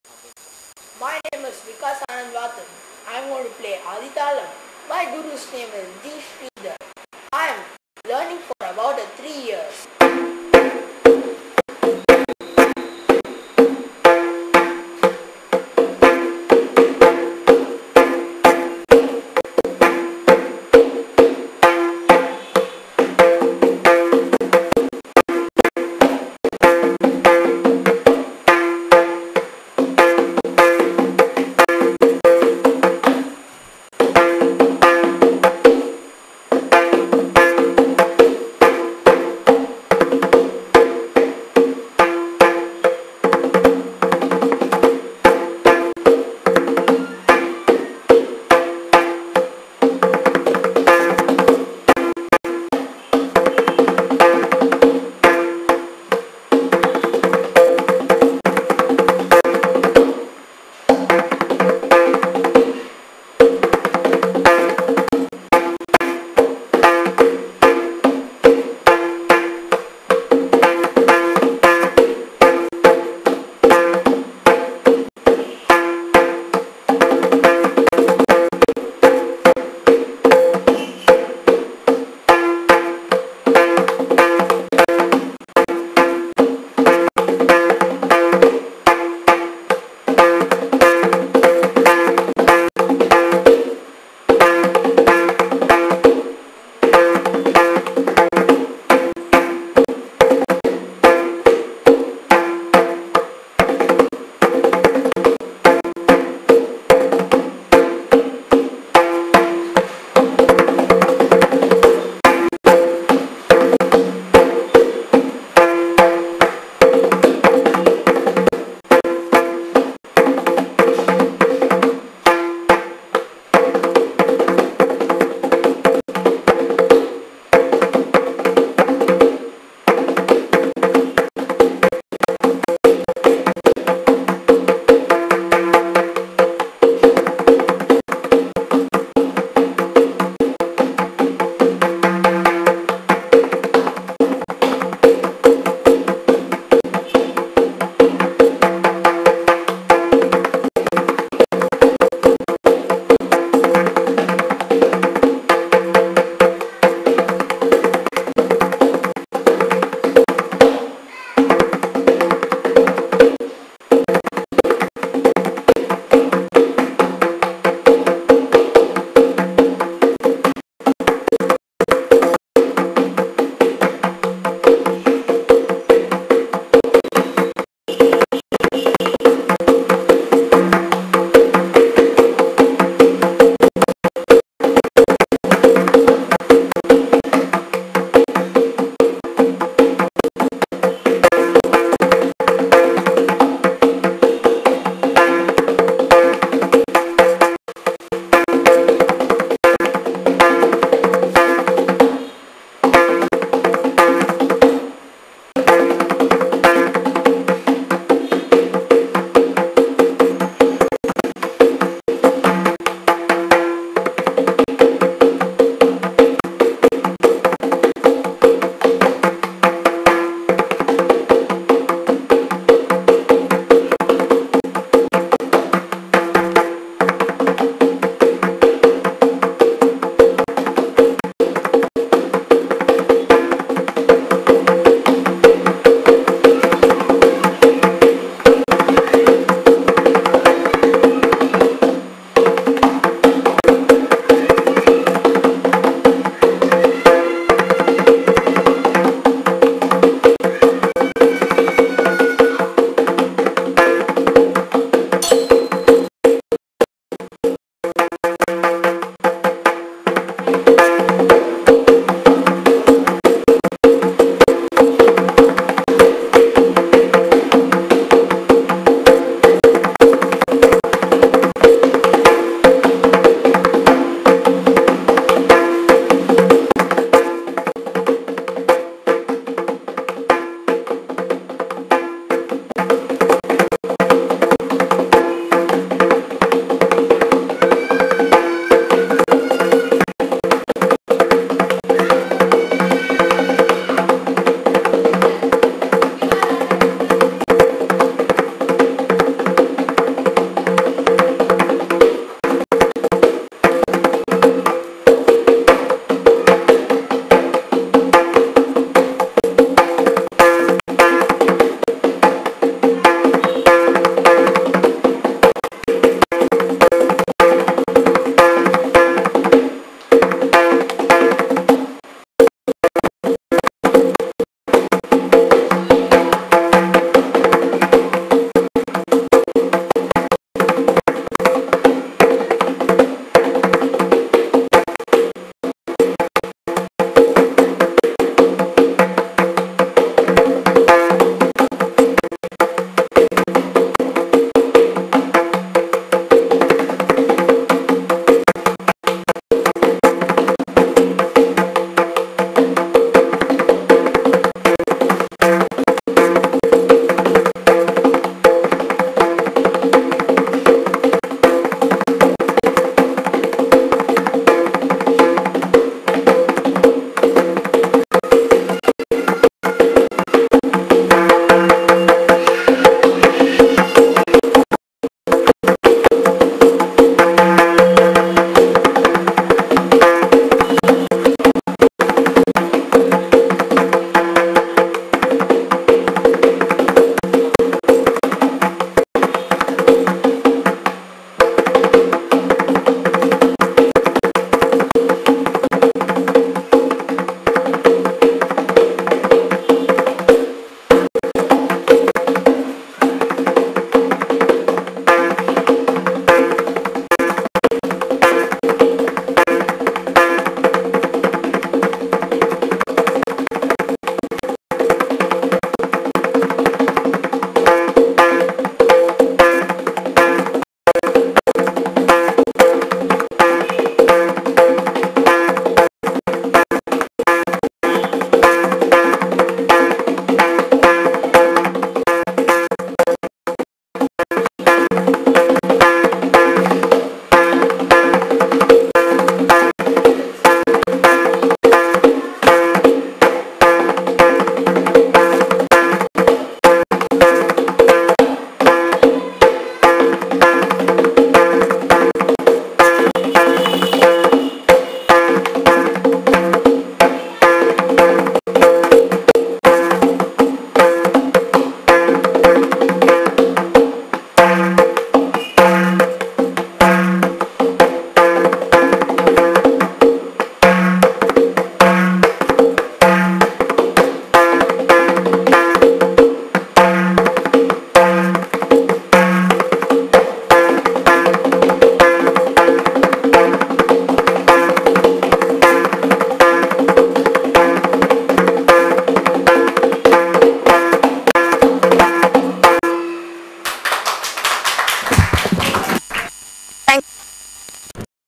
Vikas Mirudhangam Play
The play recorded on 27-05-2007at Chennai